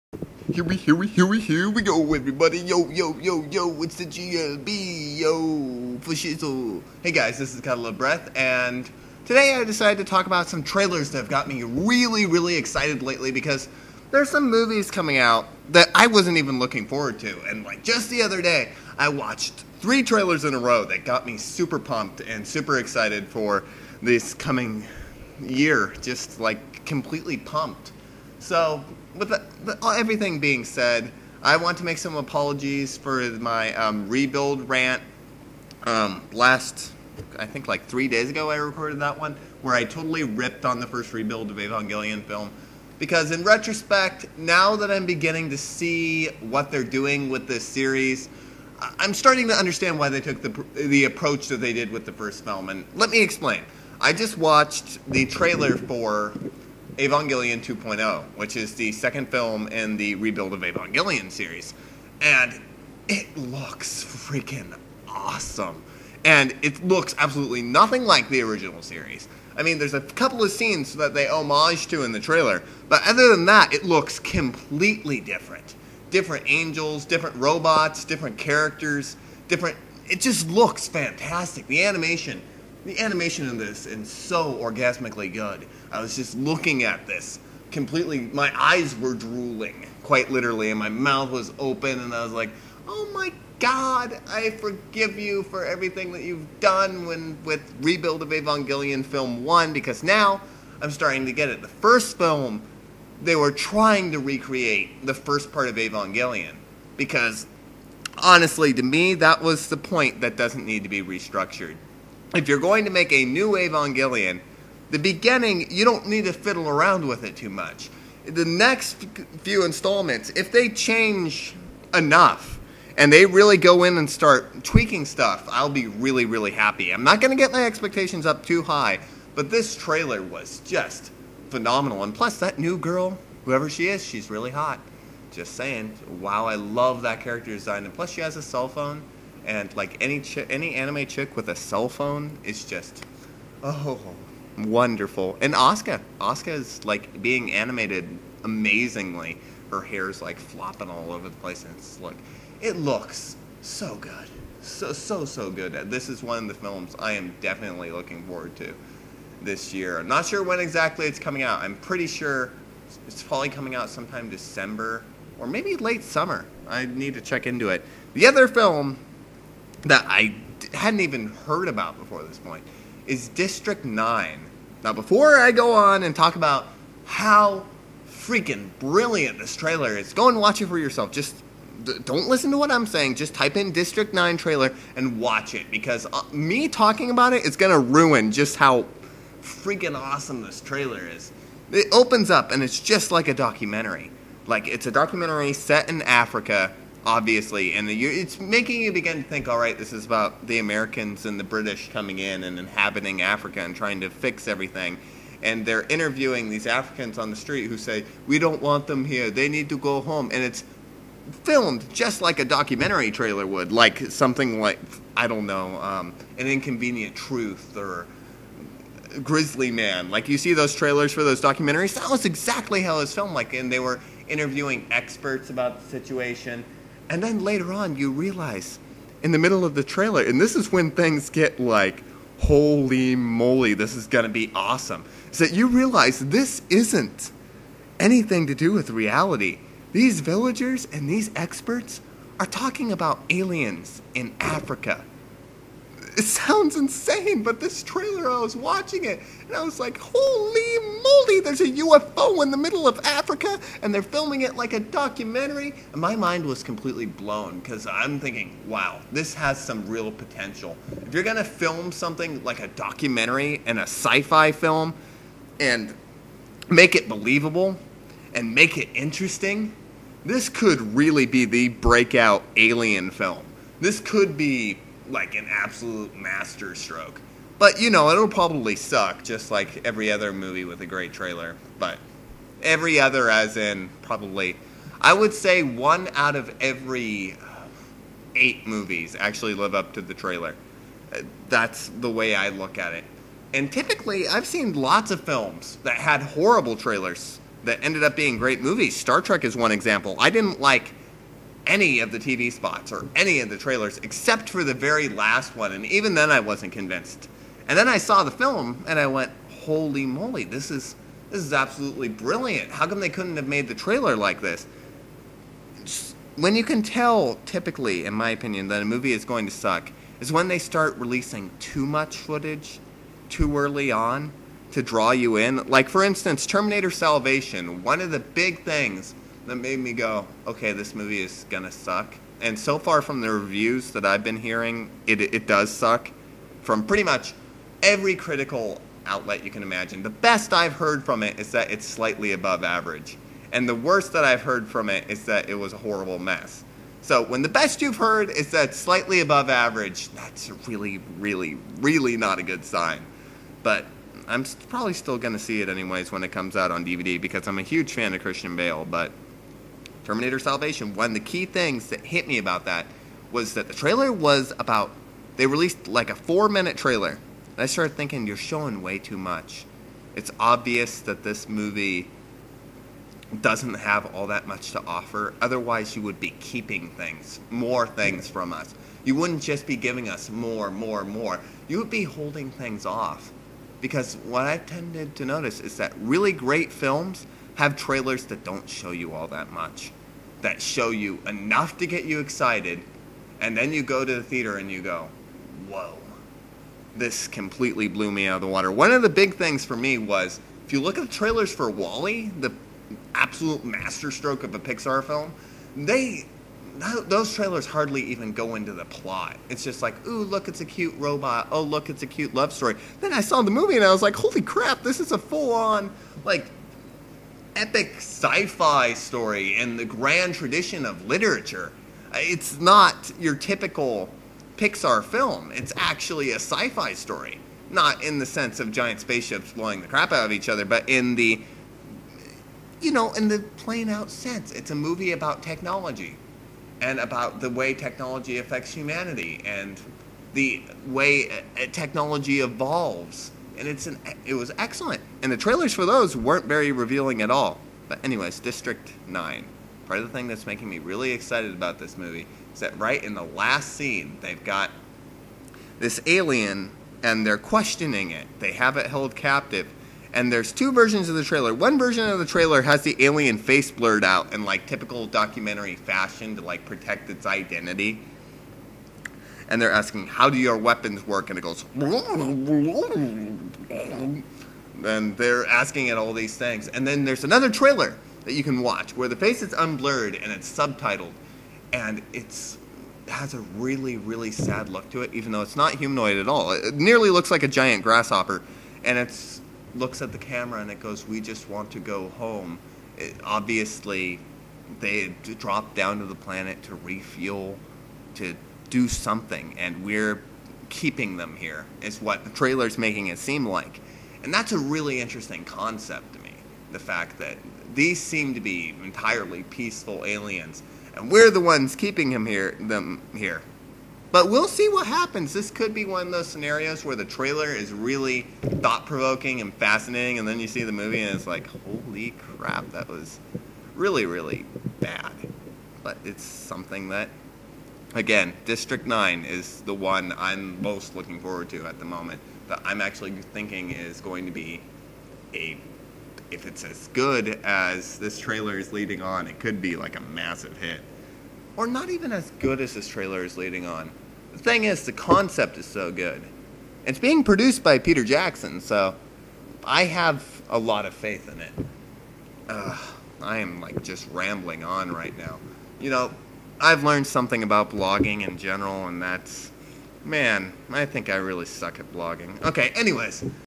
Some Trailer Impressions
I do my first impressions on Evangelion 2.0 and District 9.  Kyaaaa!  Too much... awesome.